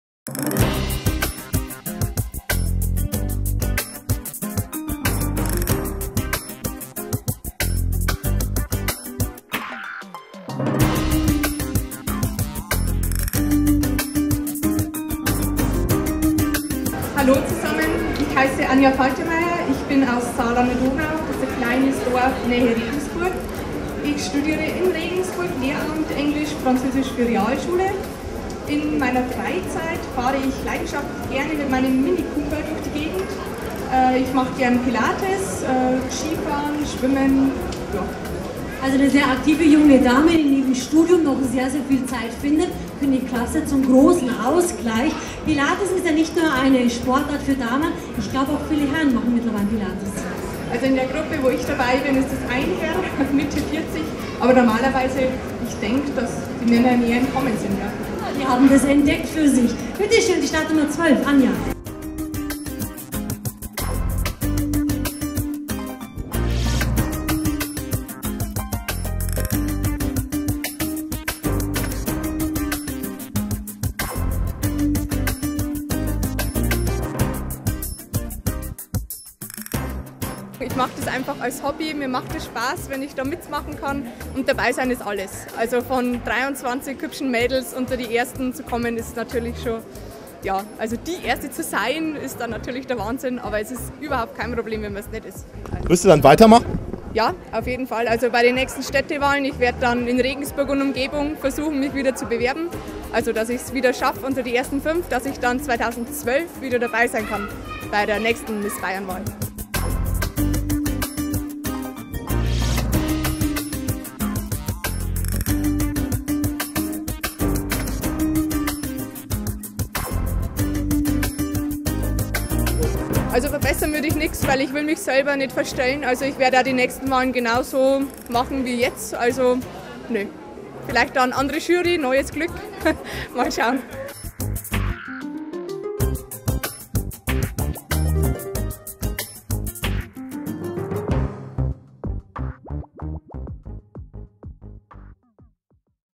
Anja Faltermeier (Miss Bayern-Finalistin) 2011, Miss Bayern Wahl im City Point Nürnberg
Lemuriafilm - Showreel - Vorstellungsvideo - Anja Faltermeier (Miss Bayern-Finalistin) 2011, Miss Bayern Wahl im City Point Nürnberg